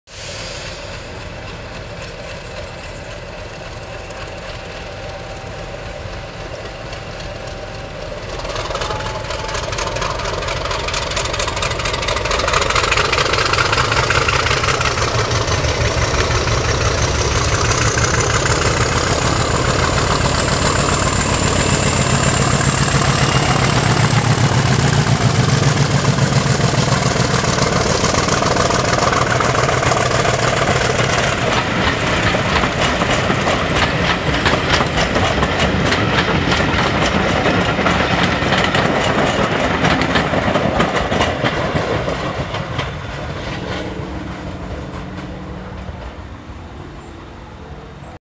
50027 departs Ropley 20 July 2013
50027 departs Ropley 20 July 2013 on the final train to Alresford. Diesel replaced steam because of dry weather and the risk of fire. I was delighted!